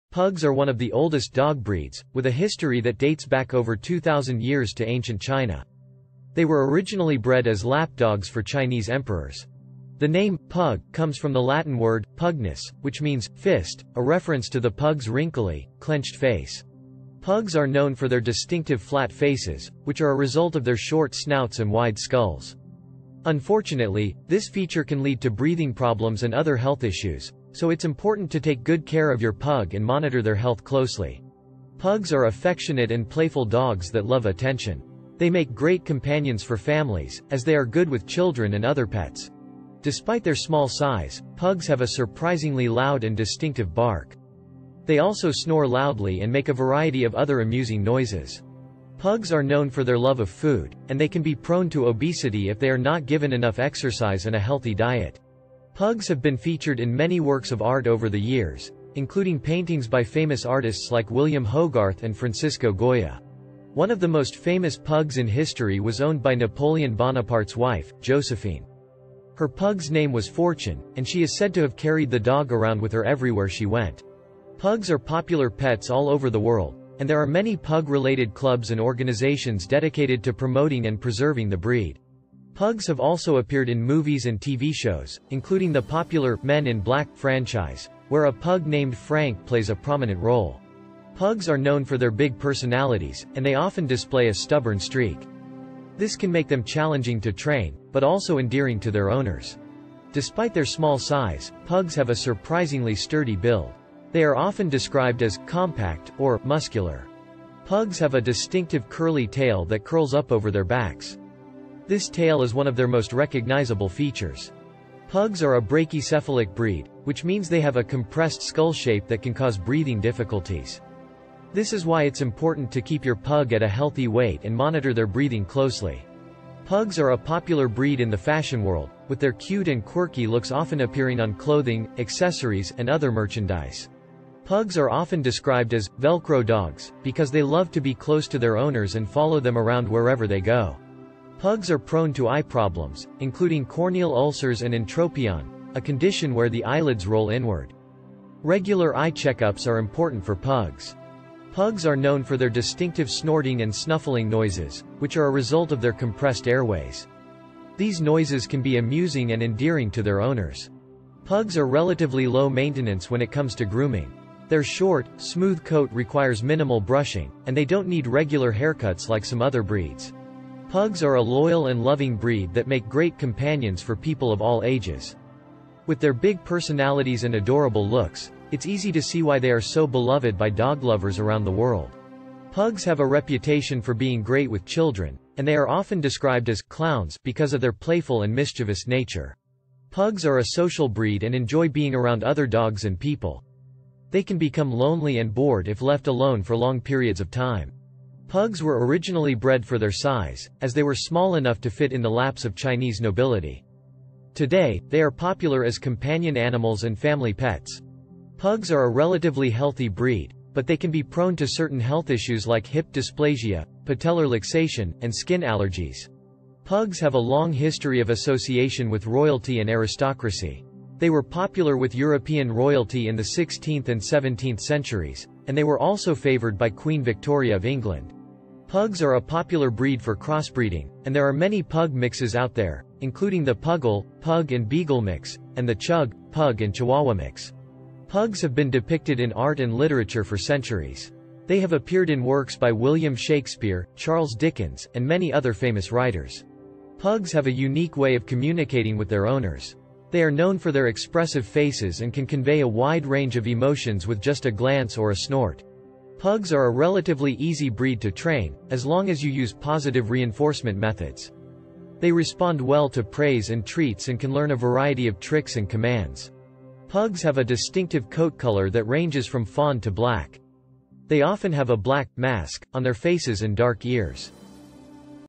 From Royal Companions to Fashion Icons: The Fascinating History of Pugs
Get ready to fall in love with the adorable world of pugs! In this video, we'll take you on a journey through the fascinating history, quirky behaviors, and lovable personalities of this beloved breed. From their surprising royal connections to their distinctive snorting and snuffling noises, we'll uncover 30 interesting and fun facts you never knew about pugs. Whether you're a lifelong pug enthusiast or just discovering the breed for the first time, this video is sure to delight and entertain you. So grab your furry friend and join us on this journey of discovery into the wonderful world of pugs!